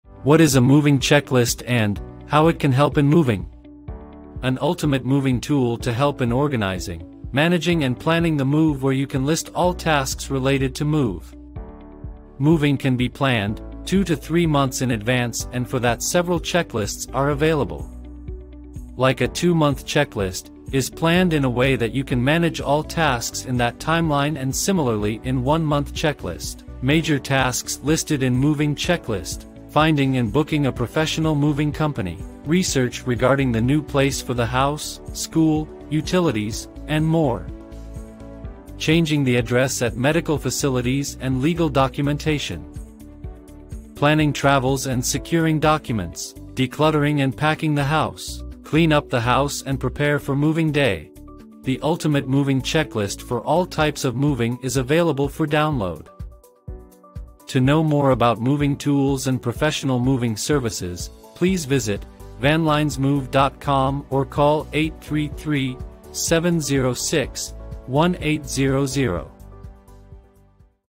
What is a Moving Checklist? How it can help in Moving | Van Lines Move
Moving Checklist Making your Move Easy:

An ultimate moving tool to help in organizing, managing & planning the move where you can list all tasks related to move. Moving can be planned 2-3 months in advance and for that several checklists are available. Like a 2 month checklist is planned in a way that you can manage all tasks in that timeline and similarly in 1 month checklist.
Major Tasks listed in Moving Checklist-
Finding and booking a professional moving company
Research regarding the new place for the house, school, utilities, and more.
Changing the address at medical facilities and legal documentation.
Planning travels & securing documents
Decluttering & packing the house
Clean up the house and prepare for moving day
The ultimate moving checklist for all types of moving is available for download